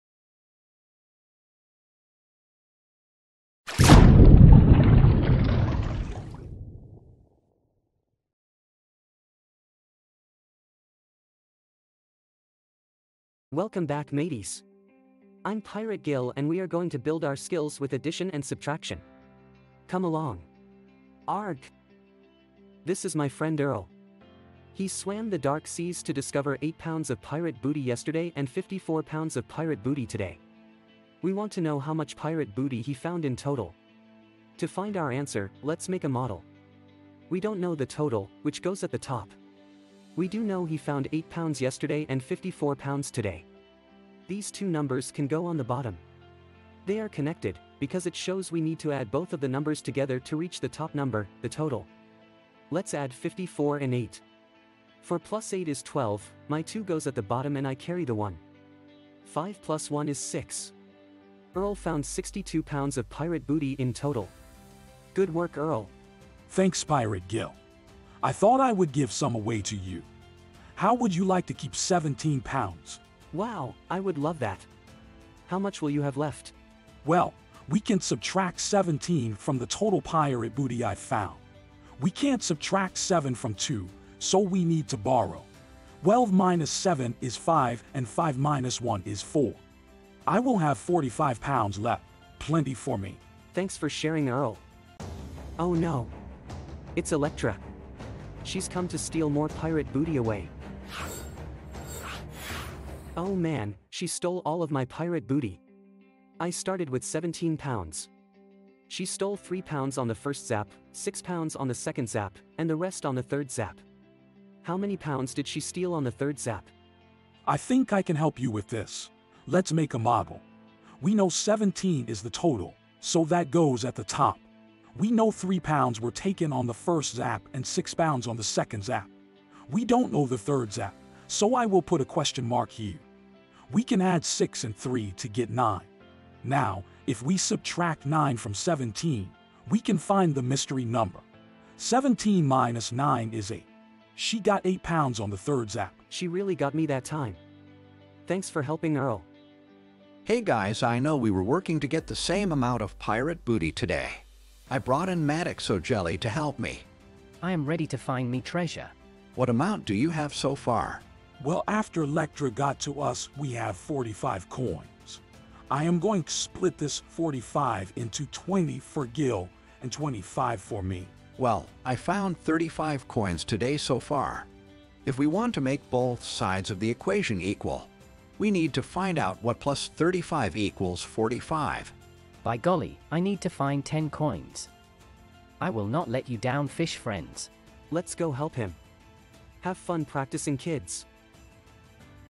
Skill Builder: Addition & Subtraction | 2nd Grade Math
Underwater Math provides engaging learning solutions for students. This remediation video supports student foundations and methods for addition and subtraction. Florida Math Standard 2.OA.1, 1a, 2